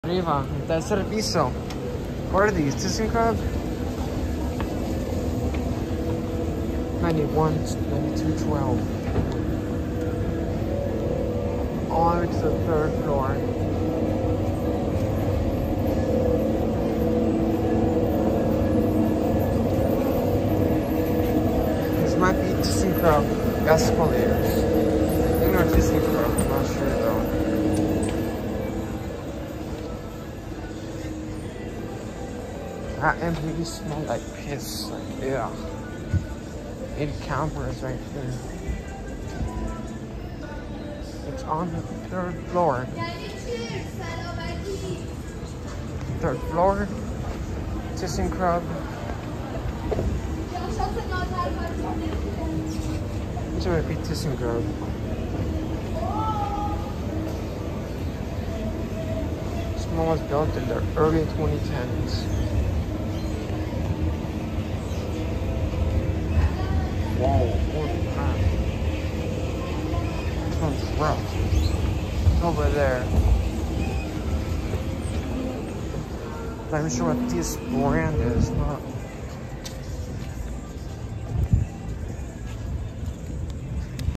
9212: Thyssenkrupp Double-File Up Escalators at Santa Monica Place, Santa Monica, CA
This was my first time filming these a set of escalators at this mall. These are Thyssenkrupp escalators from the early 2010s. This mall was either replaced or added in the early 2010a. We were on our way to The Cheesecake Factory.

Video specs:
Phone used: iPhone 15
Lens used: Standard
Microphone used: Built-in
Date of uploading: Wednesday May 1, 2024
Time of uploading: 10:50 PM
Date of editing: Thursday May 2, 2023
Time of editing: 8:24 PM
Date of posting: Thursday May 2, 2023
Time of posting: 8:24 PM
Date of filming: Sunday April 28, 2024
Time of filming: 7:58 P